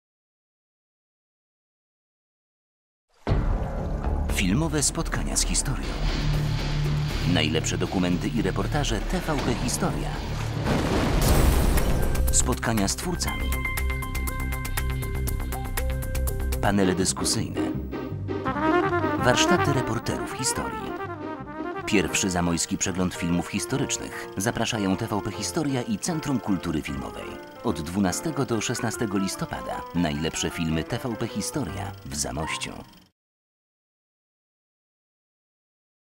1. ZAMOJSKI PRZEGLĄD FILMÓW HISTORYCZNYCH
CKF „Stylowy" w Zamościu oraz TVP Historia organizują pierwszy Zamojski Przegląd Filmów Historycznych. Na filmowe spotkania z historią zapraszamy w dniach 12-16 listopada 2012. Patronat Honorowy nad wydarzeniem objął Prezydent Miasta Zamość Pan Marcin Zamoyski.
W programie przeglądu znajdą się wybitne filmy dokumentalne i najlepsze reportaże o tematyce historycznej, spotkania z twórcami oraz warsztaty dla młodych reporterów na temat dziennikarstwa historycznego.